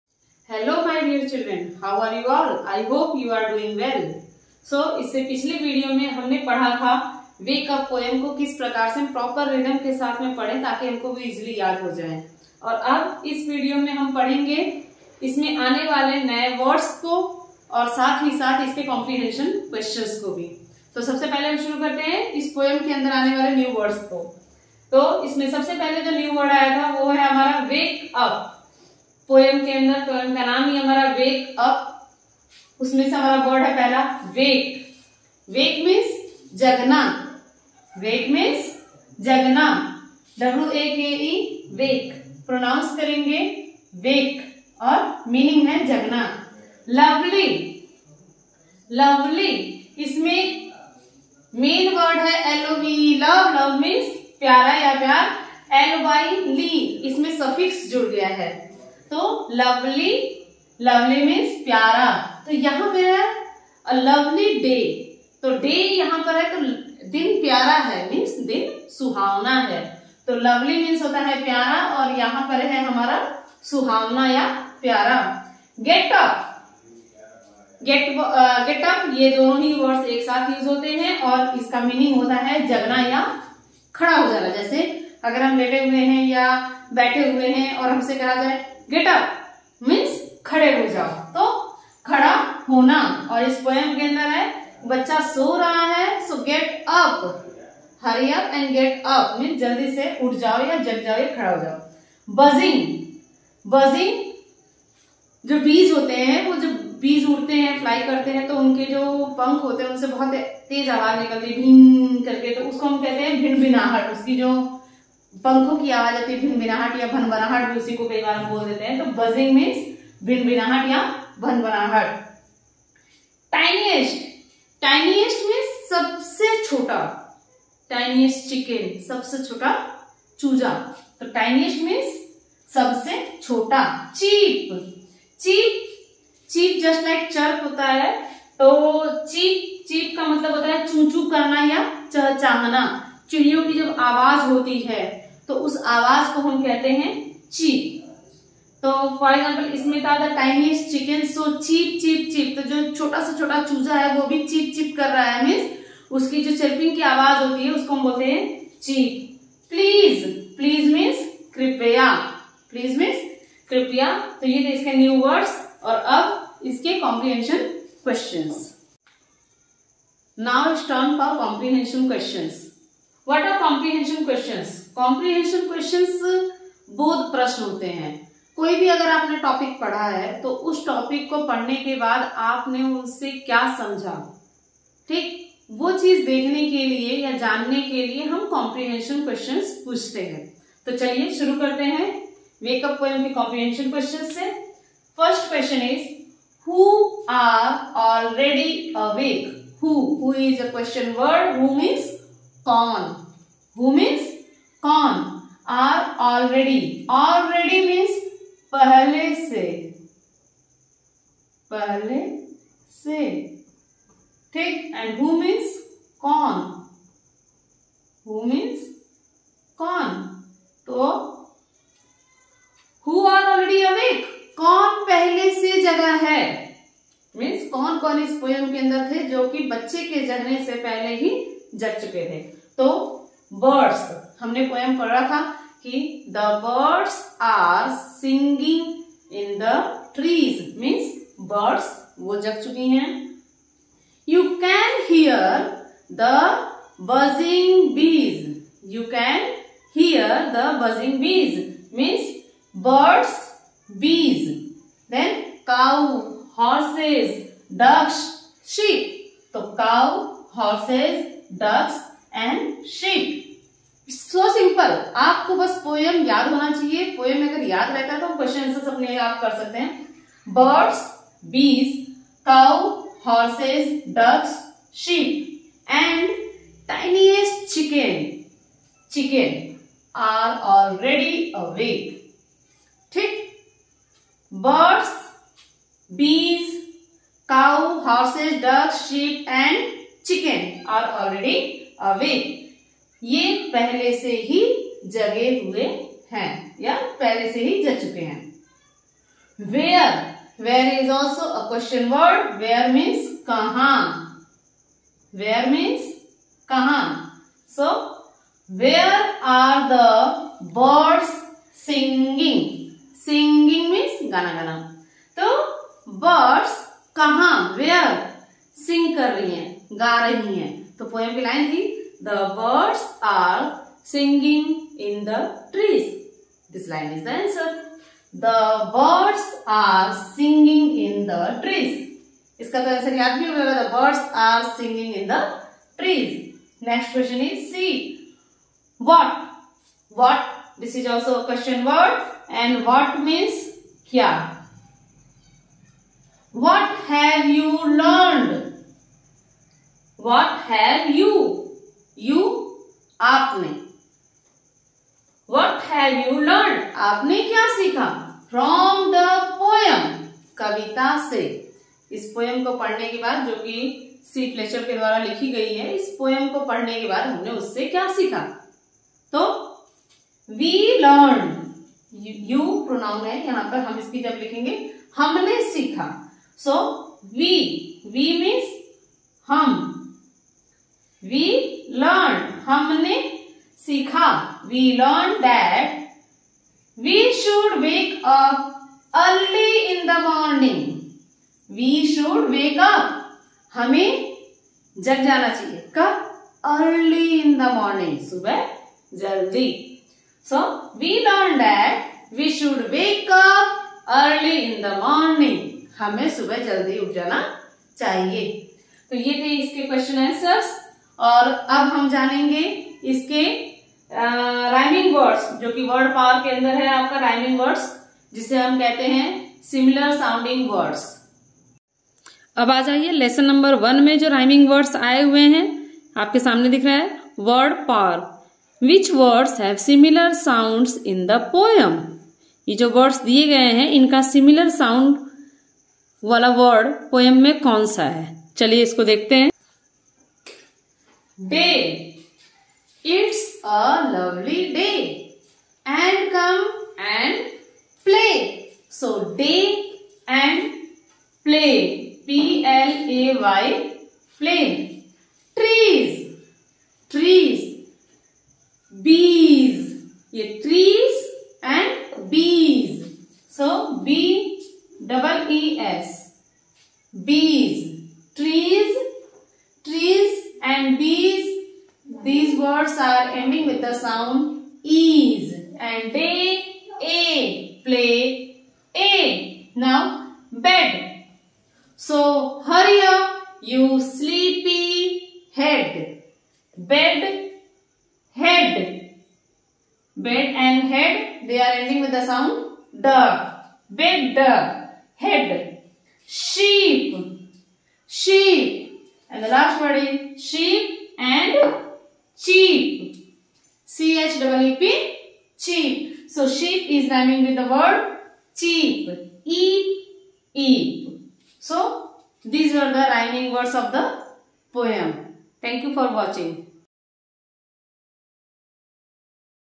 Wake up class 4 || wake up part-2 question-answers || wake up by Poonam yadav || वेक अप -2
The question answers and another exercise questions are explain in such a manner that it's very easy to understand and memorize it.